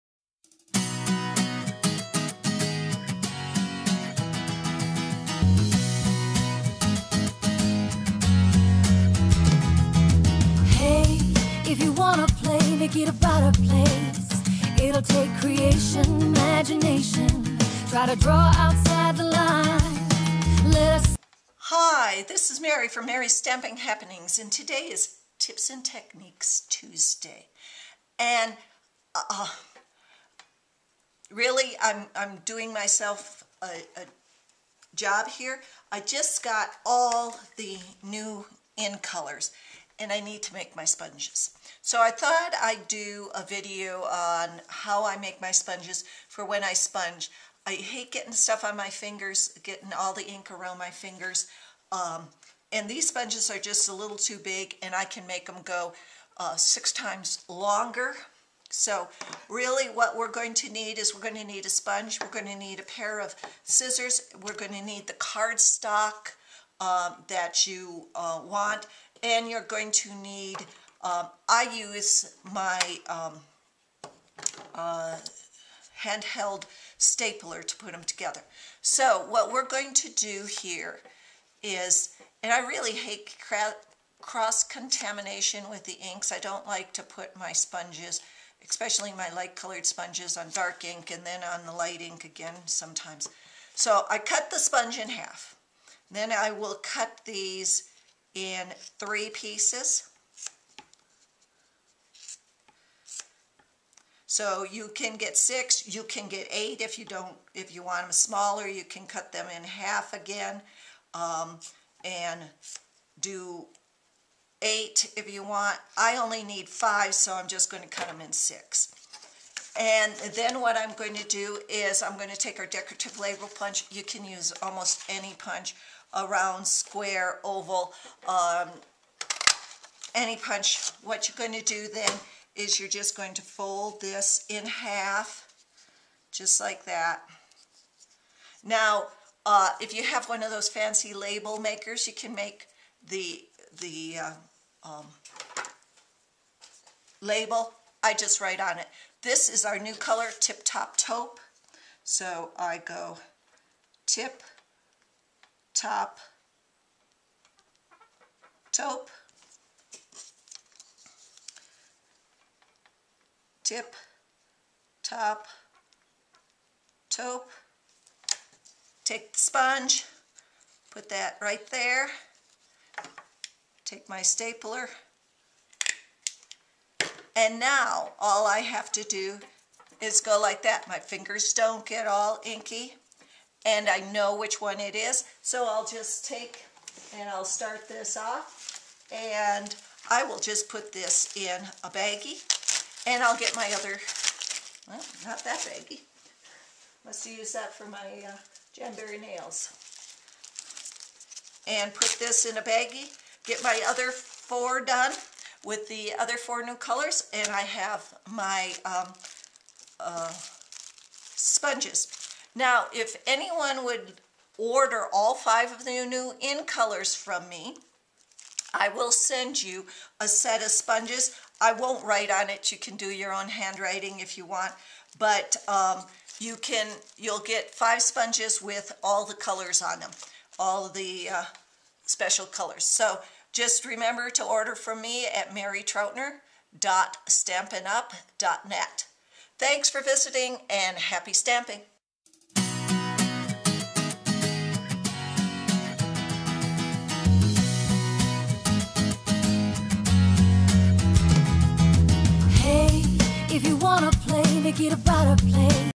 Color Coded Sponges
Tips & Techniques Tuesday is showing you how to make color coded sponges.